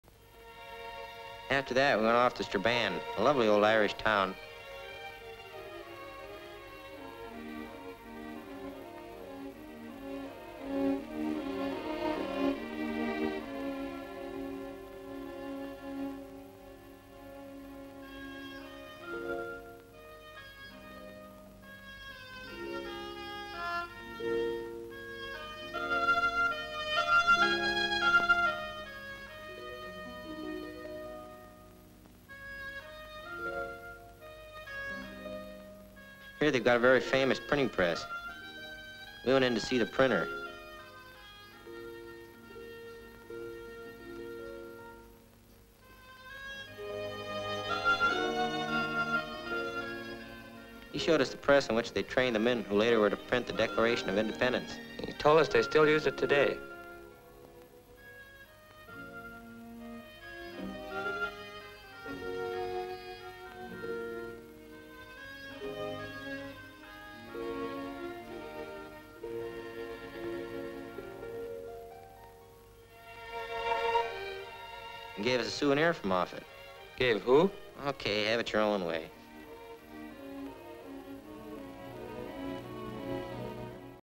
Strabane 1943
- Two American soldiers take in Strabane during the war.  They visit Gray's Printers to see presses similar to those that printed the declaration of independence.